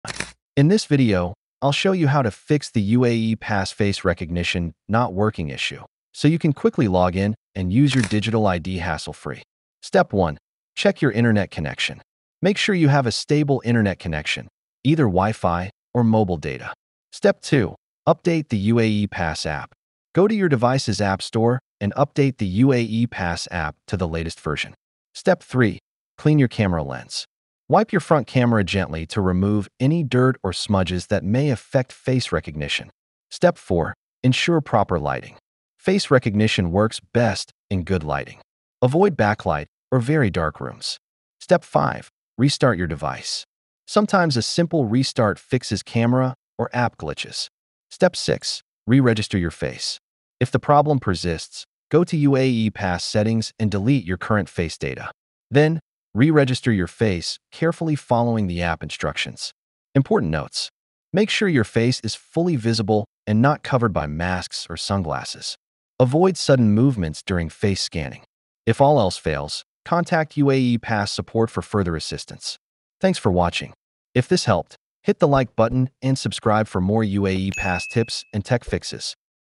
How to Fix UAE Pass Face Recognition Not Working
How to fix UAE Pass face recognition not working? If you're struggling with the UAE Pass app failing to recognize your face, especially on iPhone or other devices, this video will walk you through simple and effective solutions. Many users have reported that UAE Pass facial recognition is not working today, and this guide will help you troubleshoot it quickly.

From app permission issues to device compatibility, there are several reasons why face recognition may not work properly. Whether you're seeing general face recognition not working errors or your UAE Pass face verification is not working at all, this video explains exactly what you need to do.

If you're trying to access UAE Pass without face recognition or if you're dealing with the UAE Pass face recognition not working on iPhone, don’t worry — we cover fixes for all common scenarios. By the end of the video, your UAE Pass should be functioning smoothly again with full facial verification support.